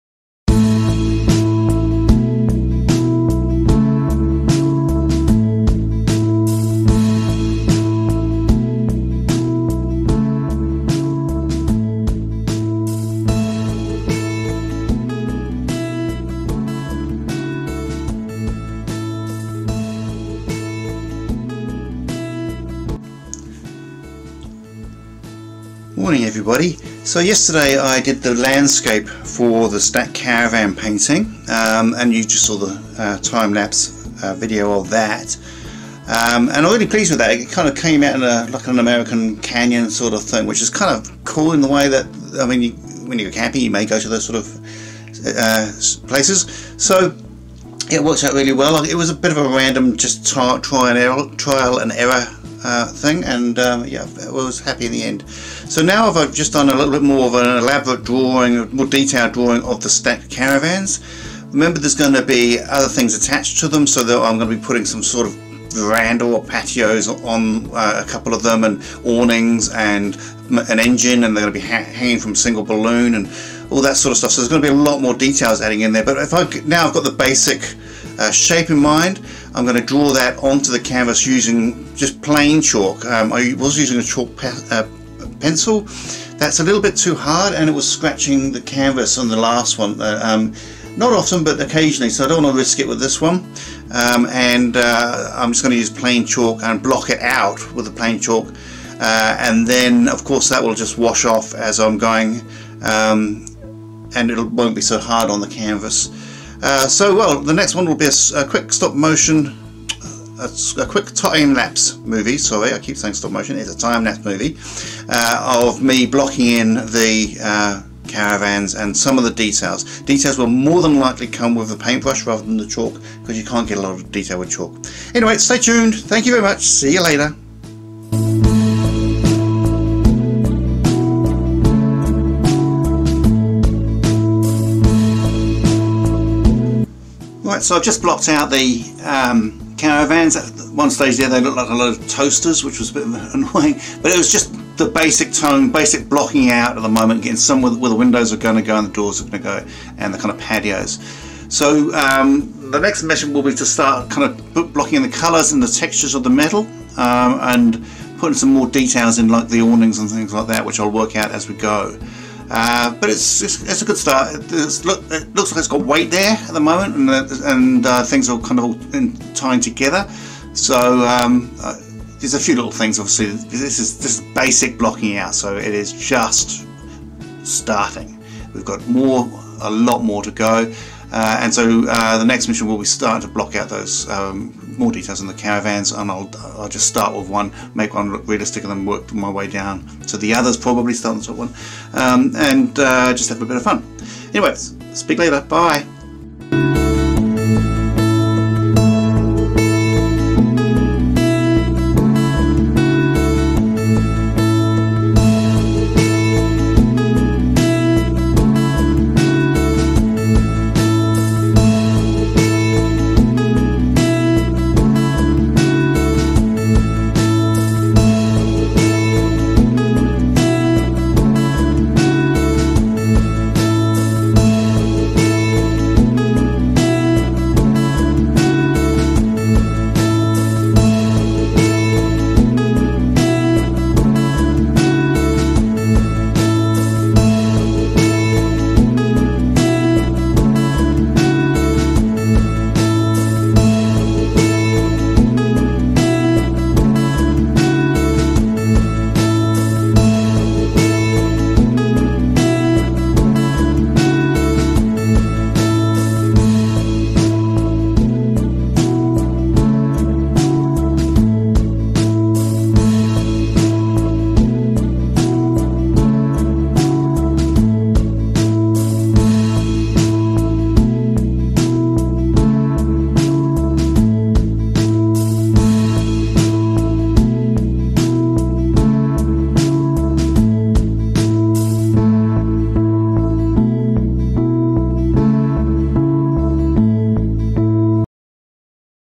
RV a painting by Ian Chapman
A painting from start to finish by Ian Chapman.
Title
RV
600mm x 900mm